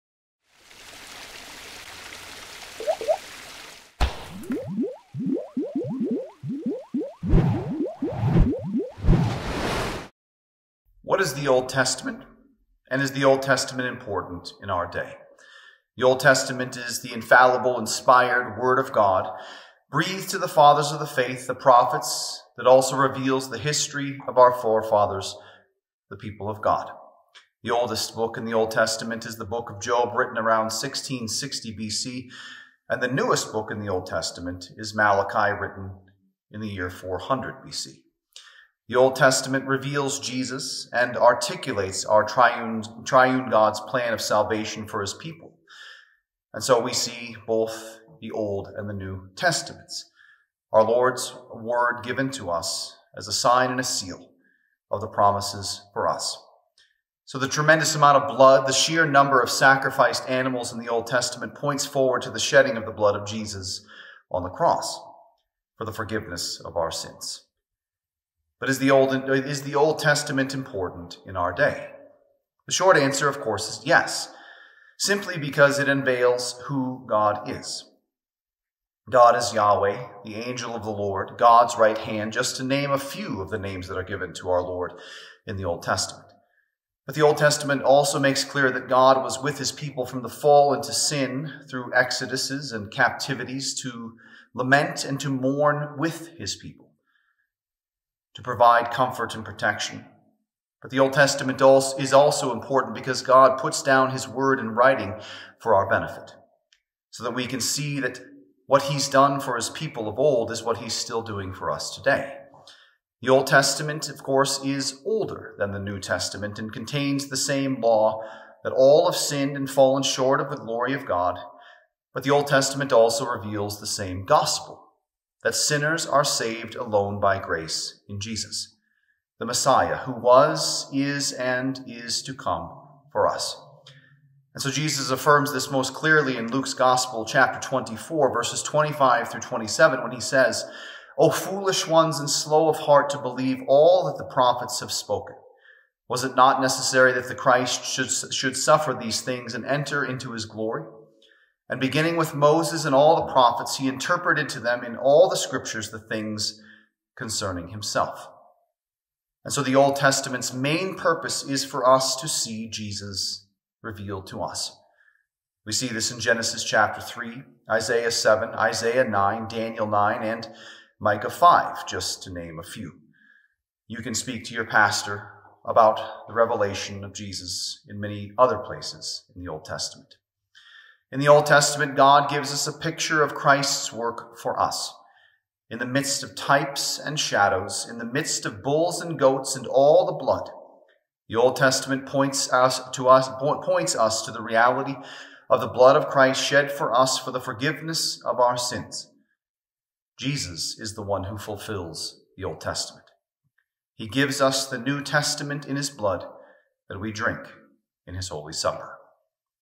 Is It Important In Our Day?
Pastor DeGroot answers the question if the Old Testament is still relevant today in our time.

Host: Rev. Adam J. DeGroot serves the Lord through the LCMS as a national (domestic) missionary  in Albuquerque, N.M.